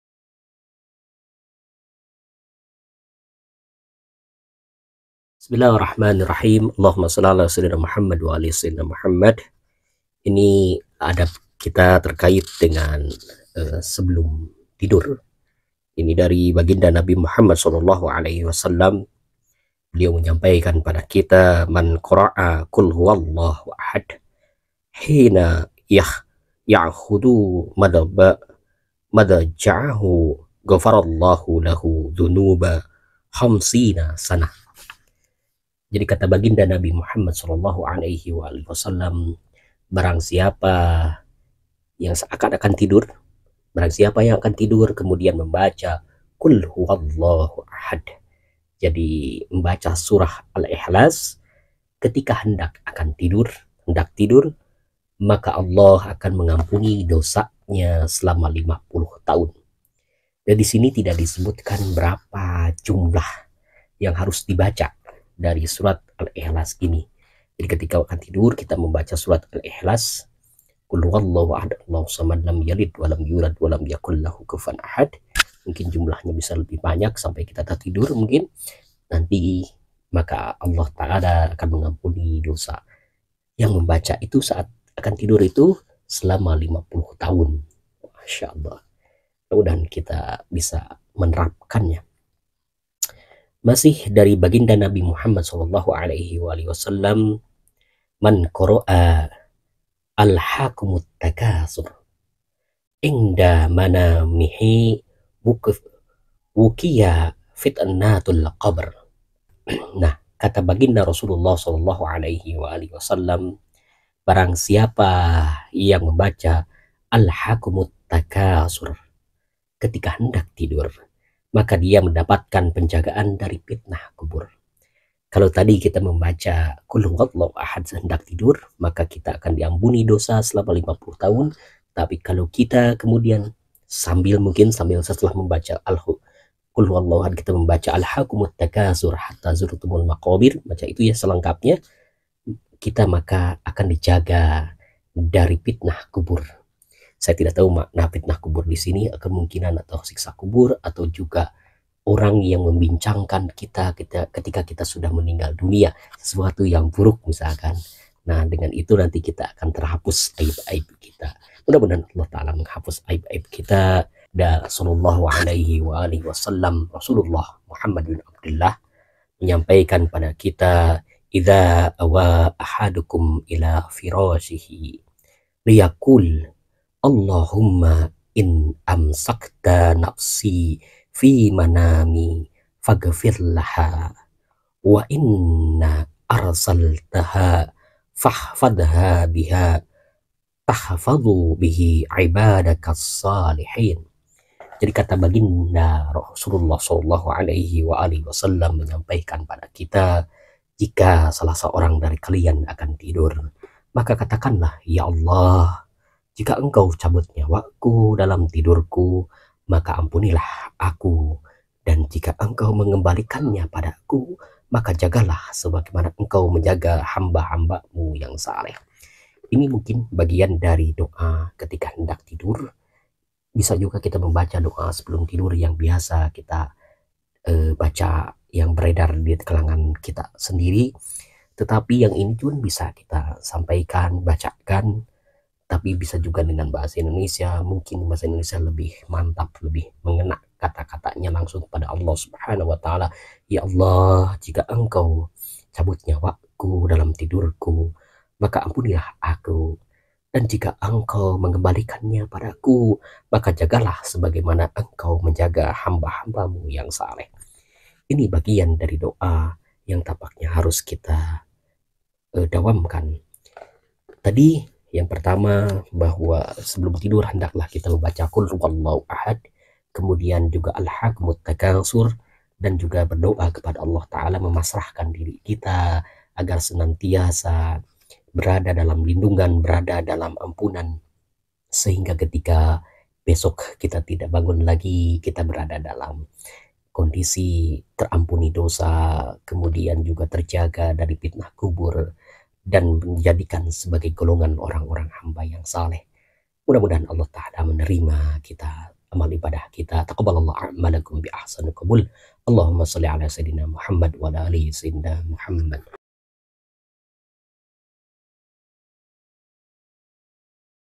Amaliah Sebelum Tidur
Book reading dari buku Muntakhab Mizanul Hikmah. Maaf jika ada salah baca dan ucap. Mohon doa untuk kami agar sehat lahir batin serta dimudahkan dalam urusan rezeki dan hidup penuh berkah.